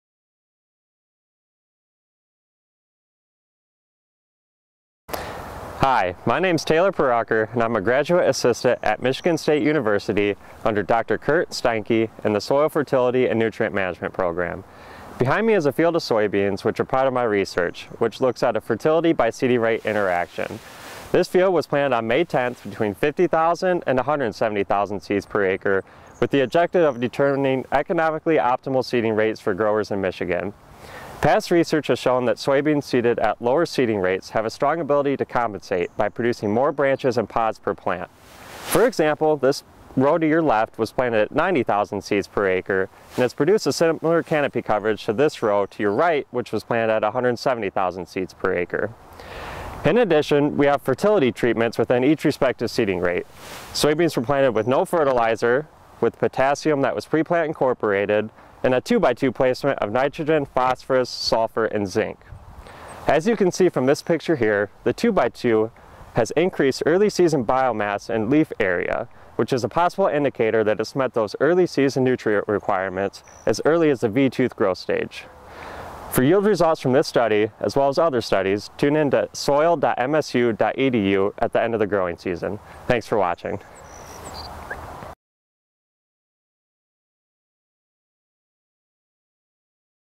Soybean Seeding Rate x Fertility Interaction Research
Learn about the interaction between various fertility management programs at multiple soybean seeding rates.